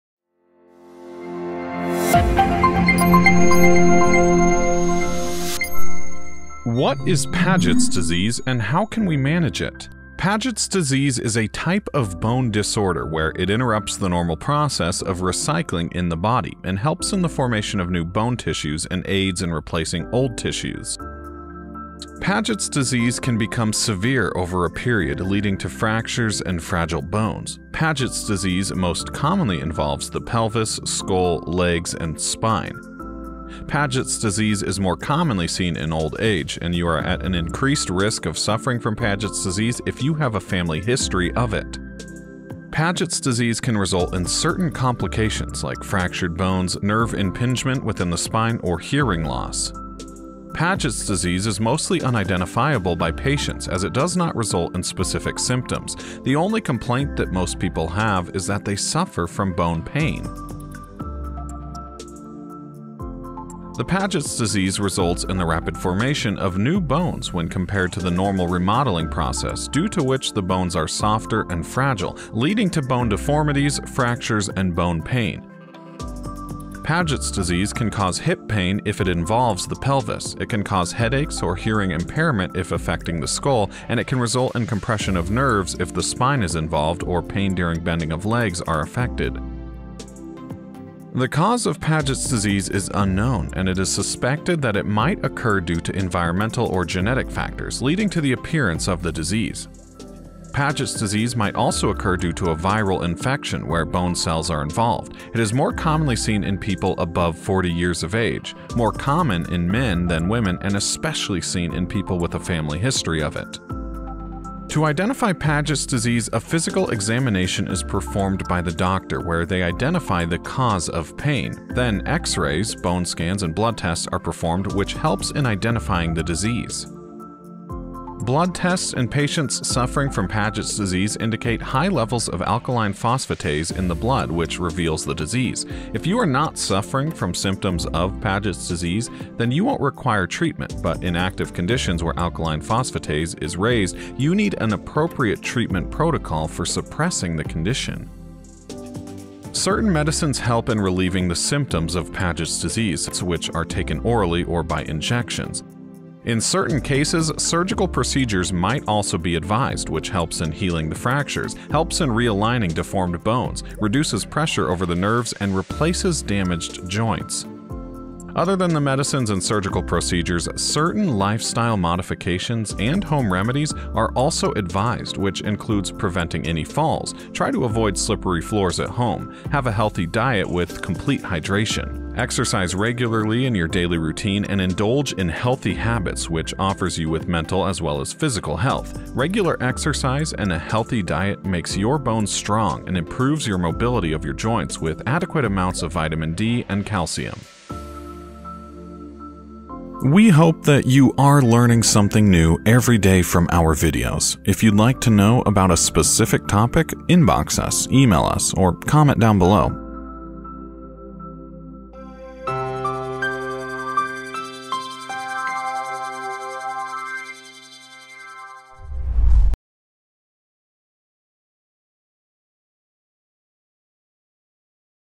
What is Pagets disease
WHAT IS PAGET’S DISEASE AND HOW TO MANAGE IT?

Paget’s disease is a type of bone disorder where it interrupts the normal process of recycling in the body and helps in the formation of new bone tissues and aids in replacing the old tissues. Paget’s disease can become severe over a period leading to fractures and fragile bones. Paget’s disease most commonly involves the pelvis, skull, legs, and spine. Paget’s disease is more commonly seen in old age and you are at increased risk to suffer from Paget’s disease if you have a family history. Paget’s disease can result in certain complications like fractured bones, nerve impingement within the spine or hearing loss.

Paget’s disease is mostly unidentifiable by patients as it does not result in specific symptoms. The only complaint that most people have is that they suffer from bone pain. The Paget’s disease results in the rapid formation of new bones when compared to the normal remodeling process due to which the bones are softer and fragile leading to bone deformities, fractures, and bone pain. Paget’s disease can cause hip pain if it involves the pelvis, it can cause headaches or hearing impairment if affecting the skull, and it can result in compression of nerves if the spine is involved or pain during bending if legs are affected.

The cause of Paget’s disease is unknown and it is suspected that it might occur due to environmental or genetic factors leading to the appearance of the disease. Paget’s disease might also occur due to a viral infection where the bone cells are involved. It is more commonly seen in people above 40 years of age, more common in men than women and especially seen in people with family history.